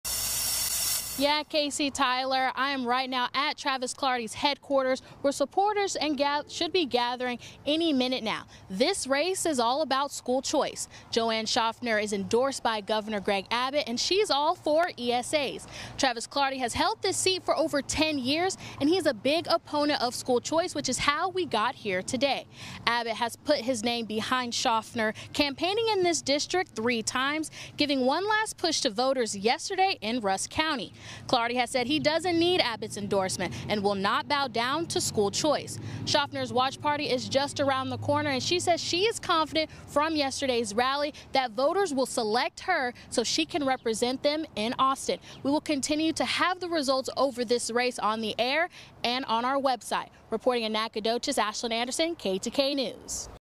KETK's Ashlyn Anderson previews District 2 election live in Nacogdoches
On Tuesday, KETK's Ashlyn Anderson previewed the District 2 election live in Nacogdoches from outside of Travis Clardy's headquarters.